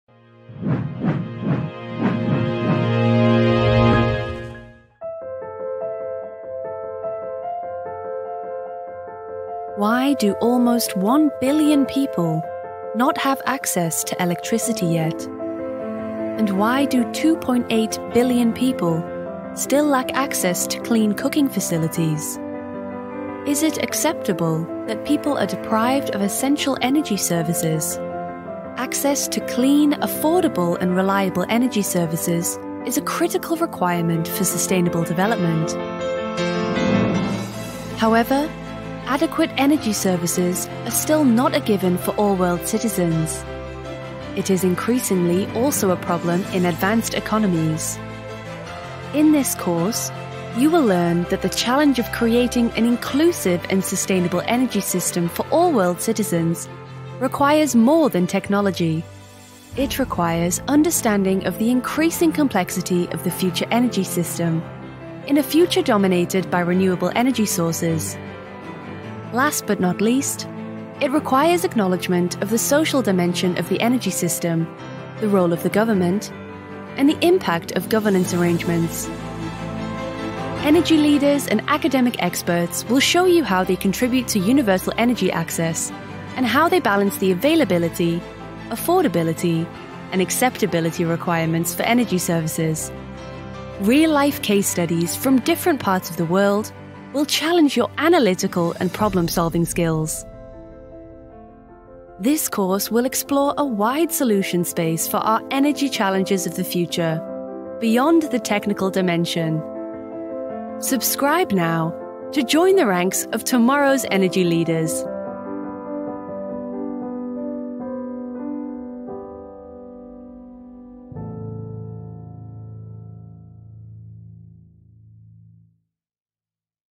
Inclusive Energy Systems - Exploring Sustainable Energy for All | DelftX
For the first time in history, the number of world citizens without access to electricity services has dropped below one billion, but still more than 2.8 billion people lack access to clean and affordable cooking fuels. Access to clean, affordable and reliable energy services for all world citizens is a precondition for the achievement of many other Sustainable Development Goals, such as health and economic development.

The provision of sustainable energy services for all is not just a technological challenge or one confined to developing countries. Industrial and post-industrial societies also need to address issues of energy poverty and energy injustice.

Rather than tackling the technological dimension of the formidable challenge to provide an inclusive energy system with renewable and climate-neutral energy resources, this course will focus on its social and institutional dimension. Introduction to the principle of the 4 As of energy services - Accessibility, Availability, Affordability, and Acceptability (environmental and social) will enrich your perspective as an engineering professional. Balancing these four critical and interdependent criteria is a recurrent challenge for individuals and society as a whole, as the characterization of the four As evolves with economic development and changing societal preferences.

You will learn how the rules of the game as defined in laws, regulation and market designs impact the balance between the 4As. Using a wider socio-technical systems perspective you will discover new solutions for the inclusive provision of energy services beyond the purely technological solutions.

After this course you can engage in a richer, more informed debate about how to achieve an inclusive energy system. You will be able to translate this knowledge into strategies to serve society’s future energy needs. The cases presented from developed and developing countries will help you to develop and test your analytical skills. Interviews with industry leaders shaping the energy system will challenge you to reflect on the position these leaders take and the interests they serve.

Lastly, you will put yourself to the test by demonstrating your newly acquired knowledge and skills as a strategic policy advisor, in writing guidelines for a strategic action plan for the energy system and institutional context which are relevant for you, in your company, your city or your country.

-Describe and recognize continuous change in (the social and technological nature of) energy systems in terms of balancing the 4 As of energy services.
-Identify key institutional characteristics of universal access to energy services.
-Determine related major implications for society.
-Analyze the security of supply and reliability of energy services in various energy systems and the related short-term versus long-term considerations.
-Evaluate the consequences of change(s) in the energy system for affordability and energy poverty, in various user environments.
-Evaluate the social and environmental acceptability of energy services provision in different economic and socio-cultural contexts.
-Value trade-offs and their legitimacy regarding the roles and responsibilities of all parties in the energy system.